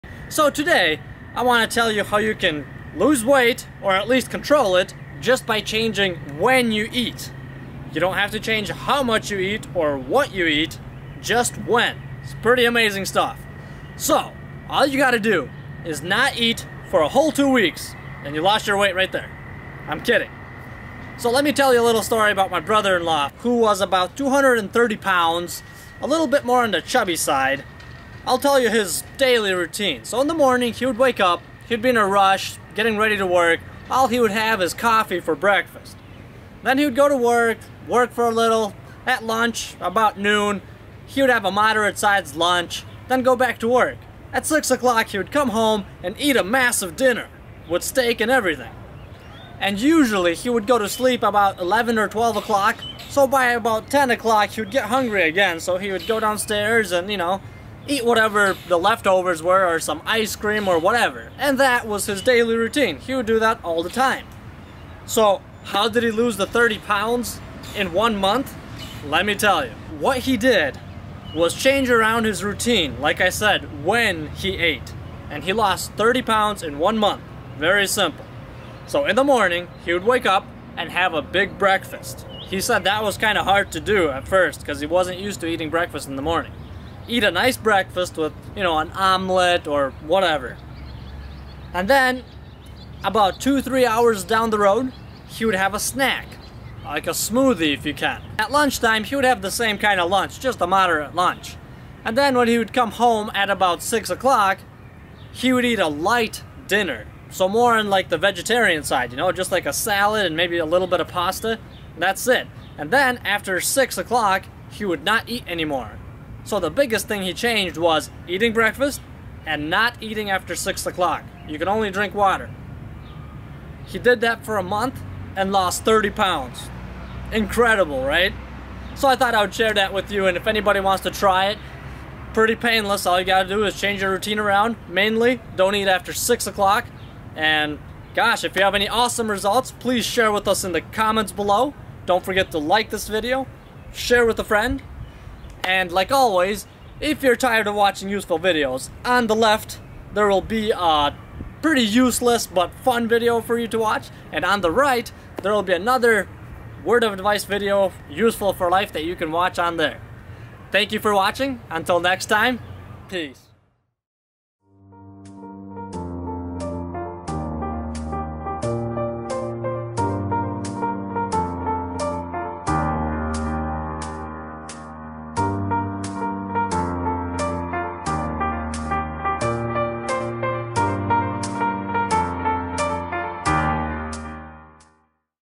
Not Eating After 6PM Results!
The results of not eating after 6pm for a month. The absolute best way to lose weight! No, its not exactly the no dinner diet.. I'm serious! Keep eating what you eat, no need to work out or go on a diet. All that it takes is changing WHEN you eat. This is an awesome little method I learned from my brother in law. He lost 30 pounds in 1 month! Just by changing WHEN he eats. Apparently the stomach's normal working hours are about 5AM-5PM. Meaning the latest you should eat is about 6. (Which is already 1 hour overtime). To sum it up, any food you throw in after 6PM will digest veryyy slowly since you are making your stomach work during it's nap time. Most of that food sits there until morning when you throw in some breakfast on top. Anyway, long story short: To lose weight, stop eating after 6PM! Also, your meals are supposed to be no larger then what can fit into both of your hands. You probably all know that eating 5 smaller meals is better than eating 3 big ones. But that's definitely a good thing to observe as well if you have the opportunity. Breakfast, Brunch, Lunch, Linner, and Dinner makes 5. ;) Not eating after 6 does not mean you can load up on pop, juice, or other yummy liquids though. If you gotta drink, then only water. This has got to be one of the best ways to lose weight! So give this a try! Make it your way of life and become a fitter, healthier person!